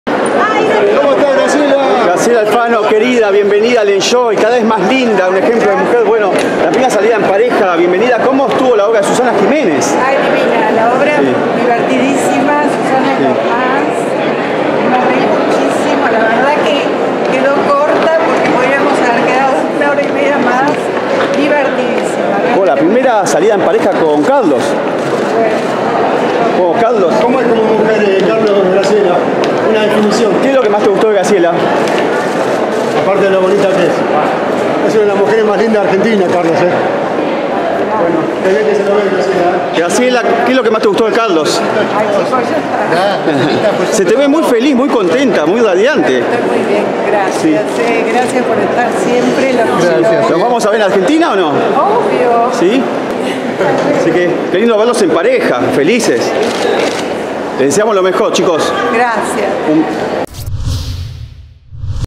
Graciela Alfano y su novio Carlitos fueron a ver a Susana Giménez. @EnjoyPDE
El único portal de espectáculos en realizar producción audiovisual propia.